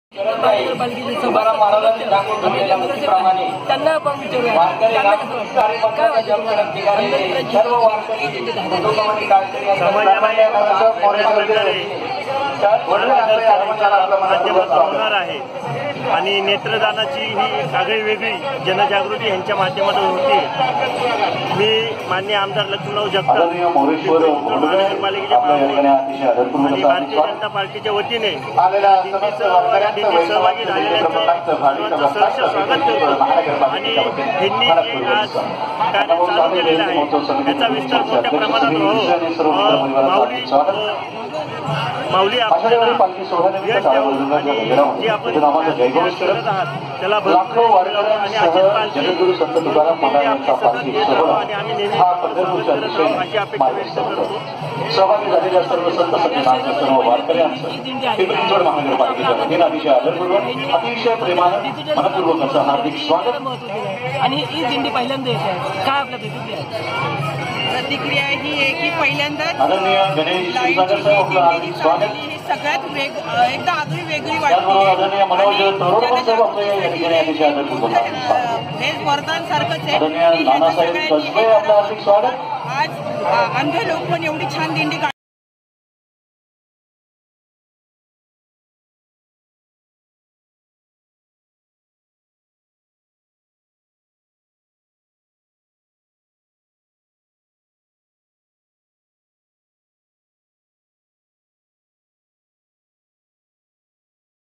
Satish Navle interviews Nagarsevika-Madhuri Kulkarni and Nagarsevak-Vijay Shinde
Live for Braillevani Radio from Bhakti Shakti, Nigdi